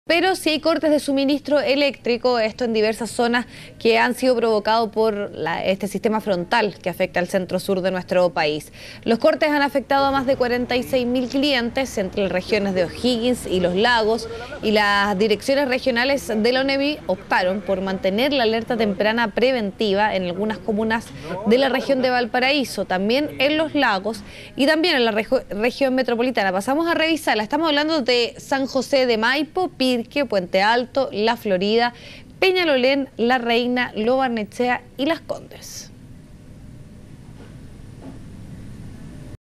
Sistema frontal en el centro sur del país provocó cortes de suministro elétrico en diversas regiones
Esta situación afectó a más de 46 mil clientes entre las regiones de O'Higgins y Los Lagos.